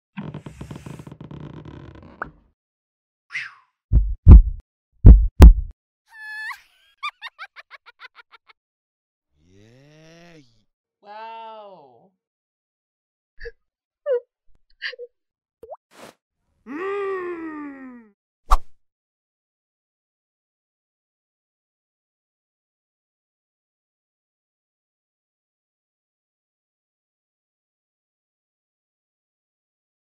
Facebook Reactions
E se os botões de reação do Facebook fossem animados?